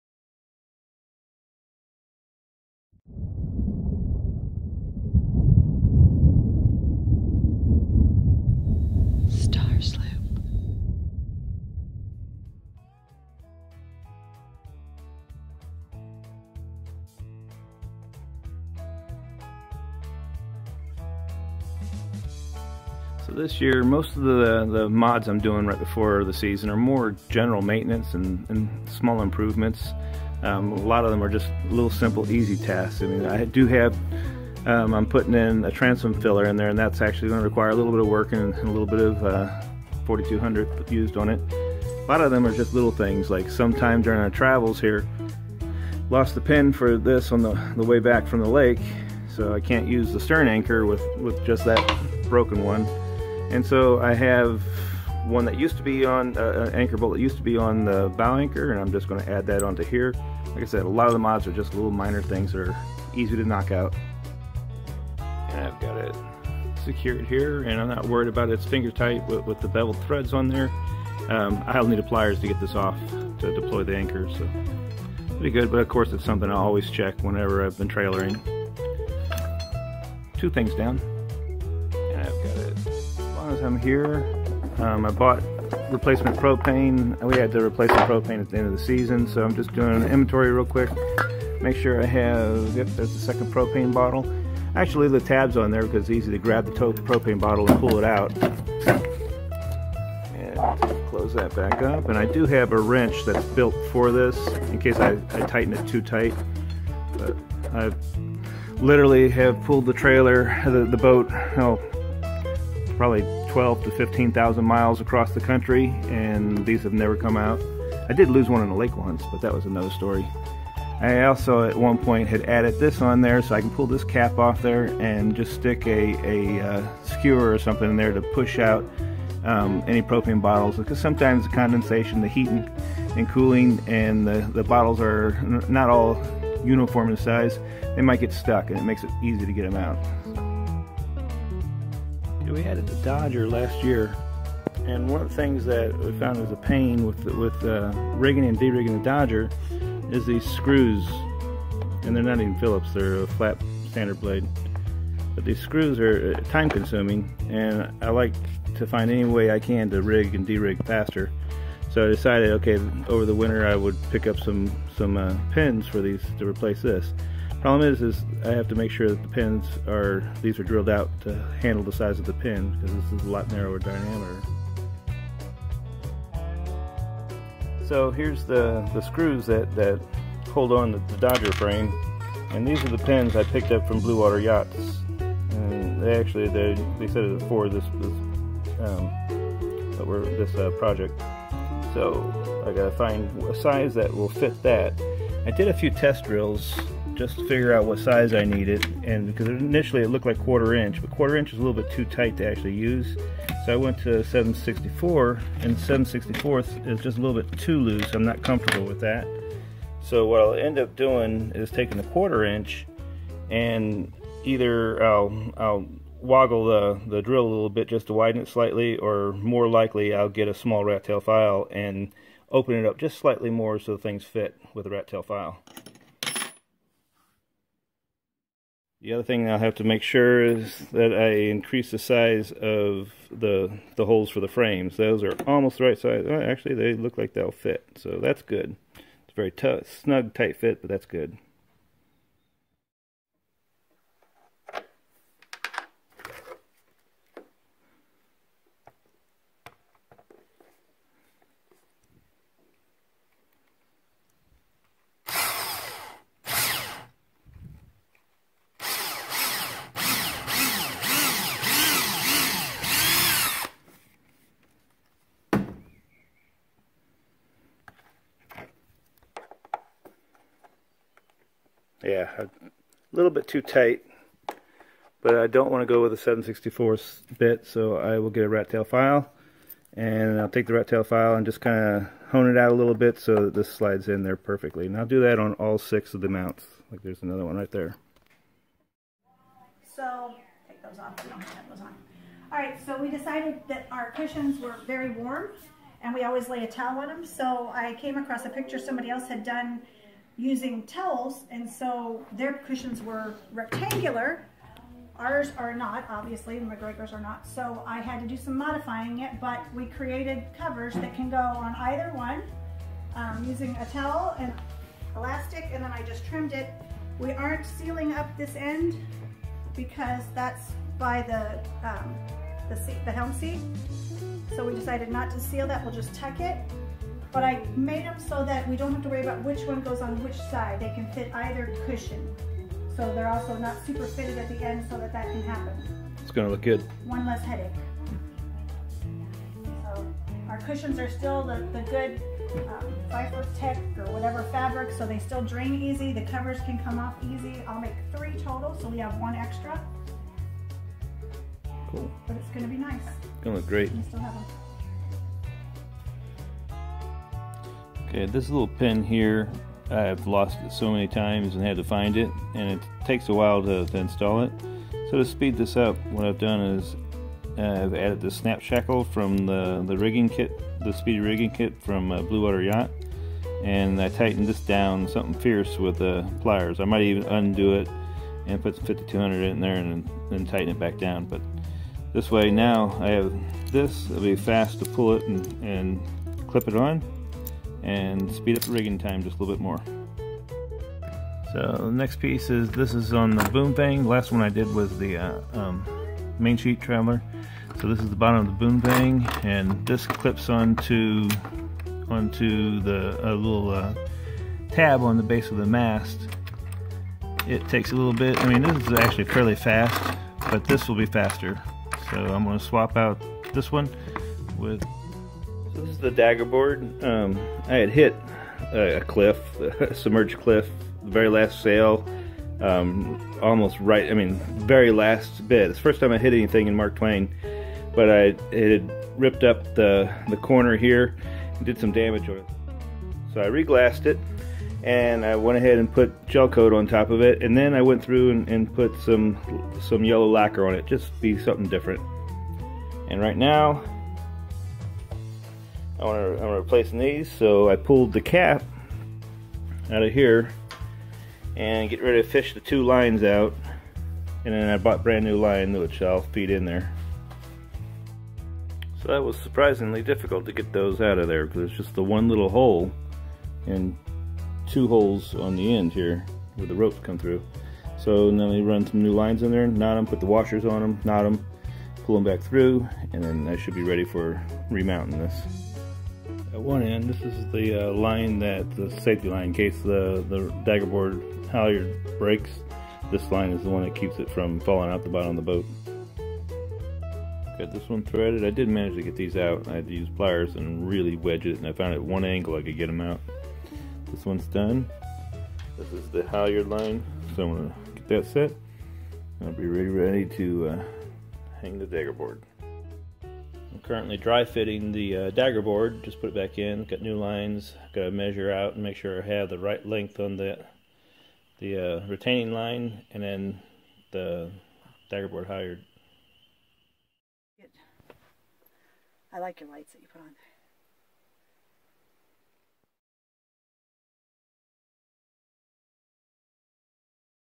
2017 Mods & Maintenance Part 2 on our MacGregor 26M - Galactica
Second video for the 2017 season (2017v2) showing the a number of modifications and repairs including speed rigging tricks, daggerboard repair, new lines, bolt replacements, new cockpit cushion covers, etc..   Second of two mod videos.